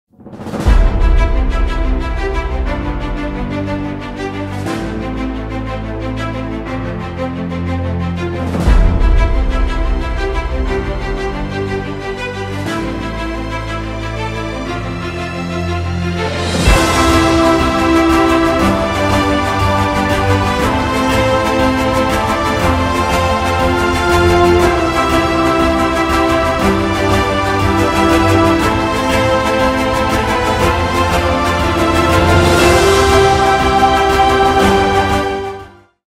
Zaggle Propel: The ultimate incentivization program for your business
Zaggle’s Propel is a one-stop solution to transform incentivization of ChannelPartners.
Driving loyalty, Revenue and Profits has been made super-easy with its competitive features.